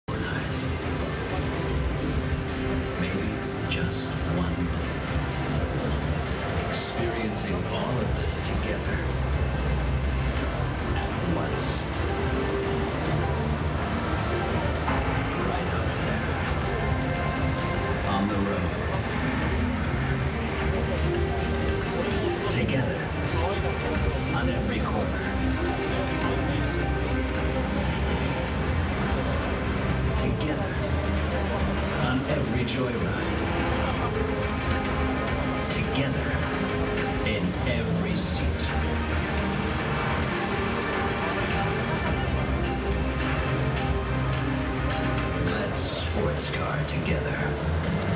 ポルシェ 新型パナメーラ 4 E ハイブリッド スポーツツーリスモ PORSCHE Panamera 4 E-Hybrid Sport Tourismo：東京モーターショー2017
2017年10月30日に東京ビッグサイトで開催された東京モーターショー2017、一般公開日3日目、ポルシェブースで展示されていた新型パナメーラ 4 E ハイブリッド スポーツツーリスモ。